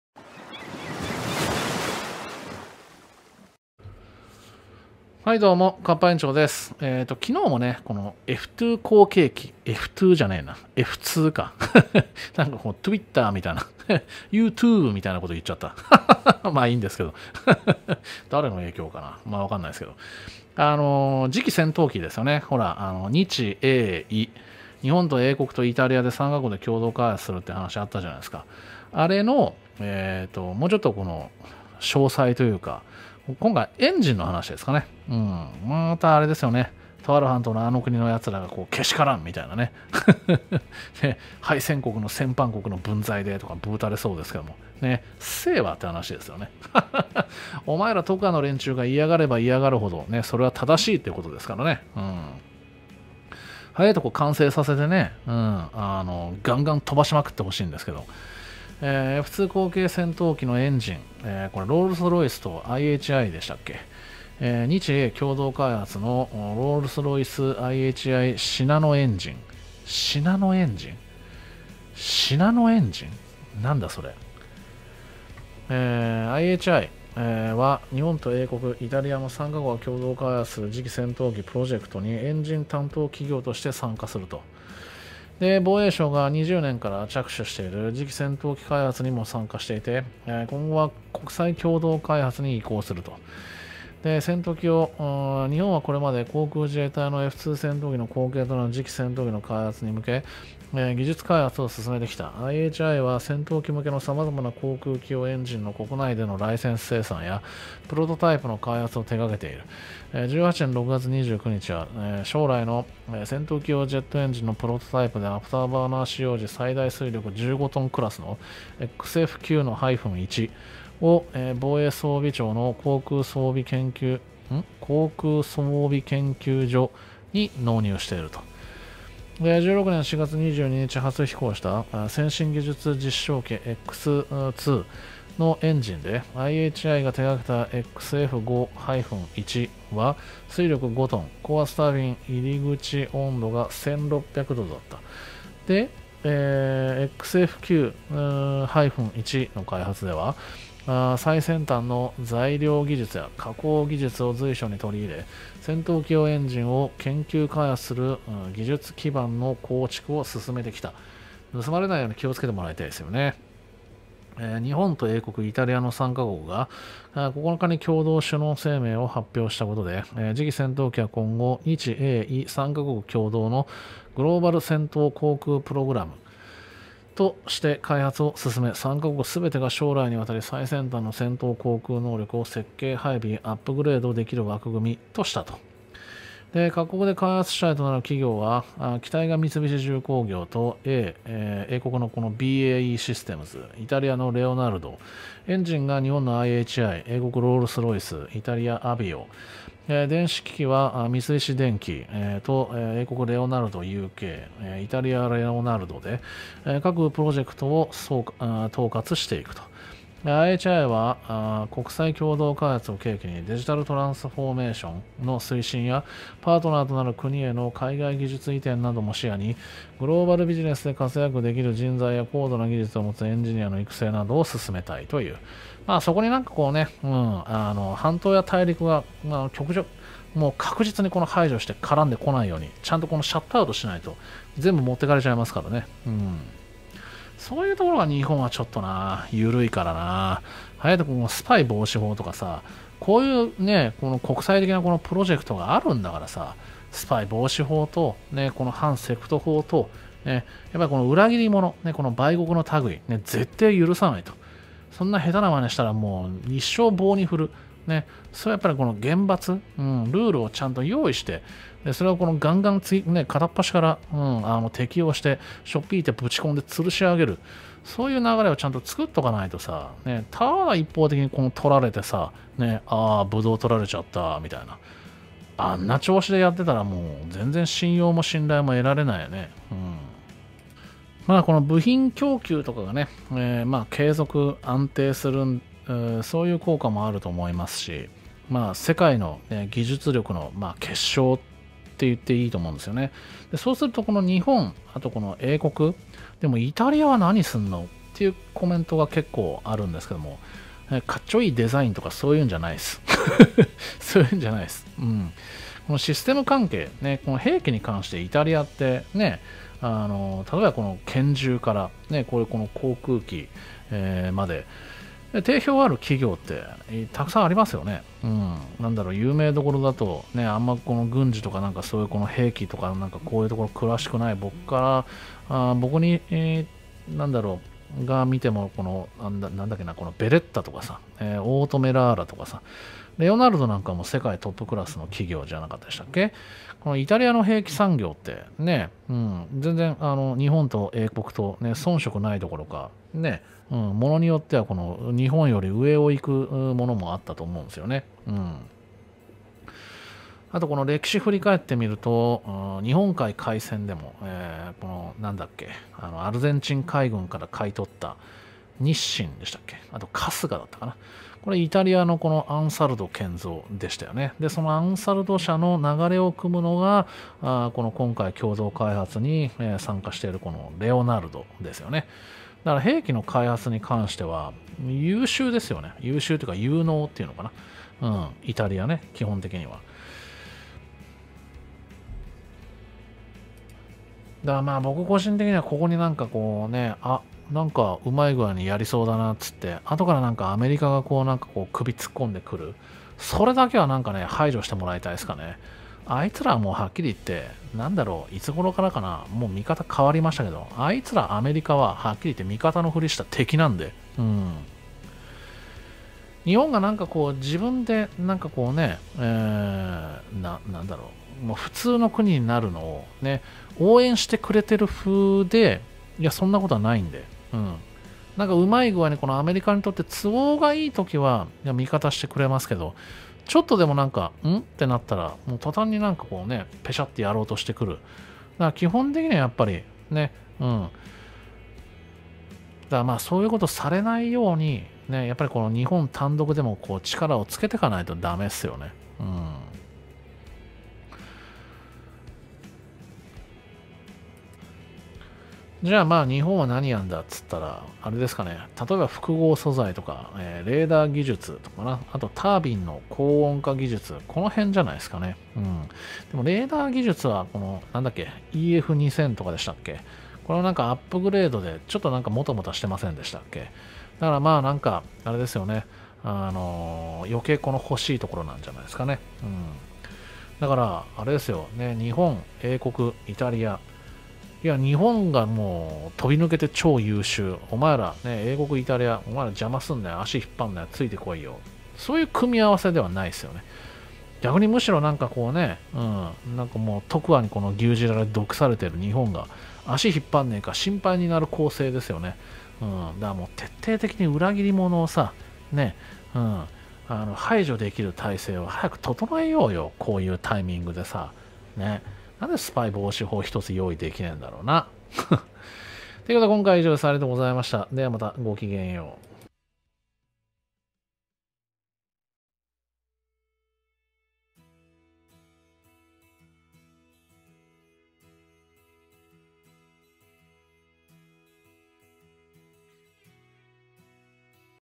F2後継戦闘機のエンジン、日英共同開発のロールス・ロイスIHIシナノエンジンを搭載⇒某国が猛烈に不満を表明中
Aviation Wire